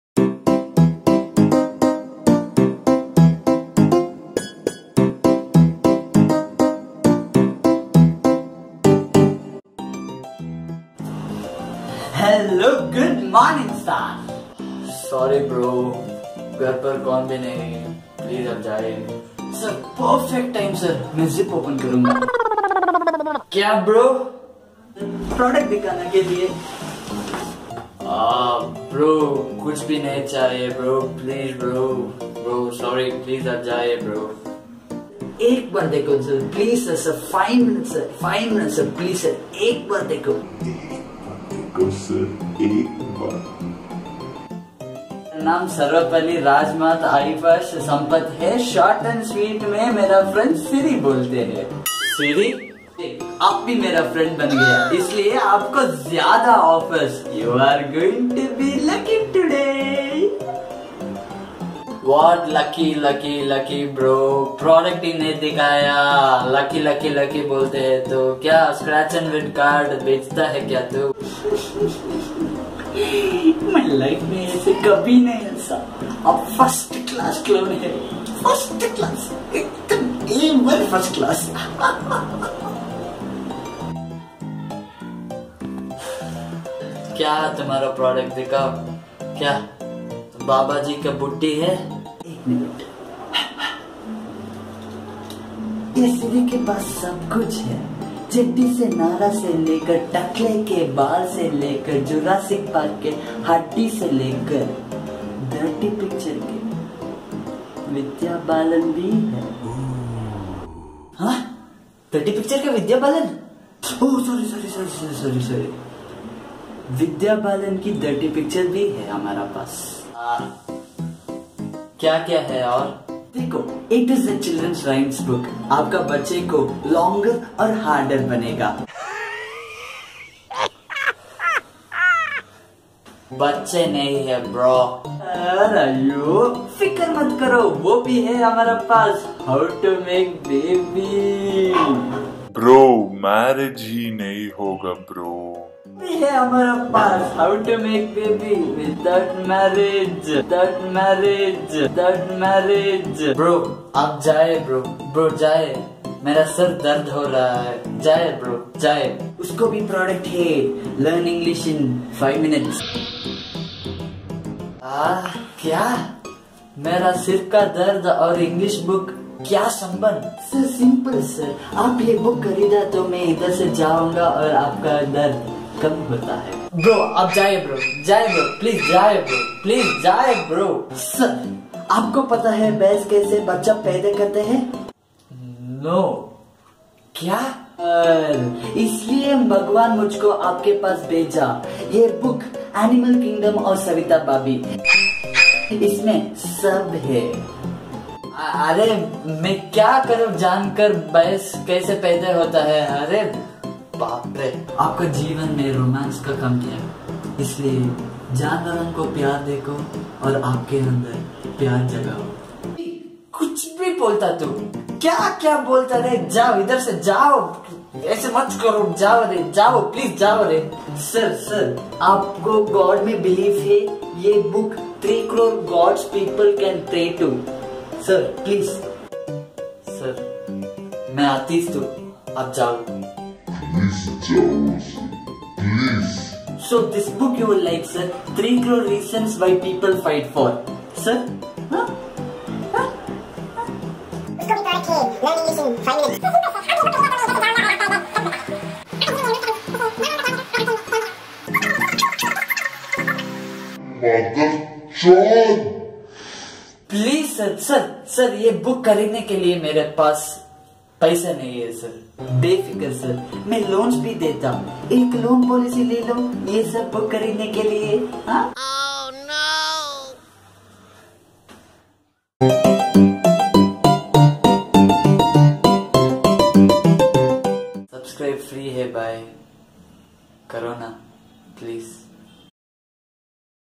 THE SALESMAN | Indian comedy | MUST WATCH !!
We have all at one point of our lives come in contact with this race of humans called " salesmen" .It is in their blood to never give up . Here is a funny comedy sketch of how annoying a salesman can become at times .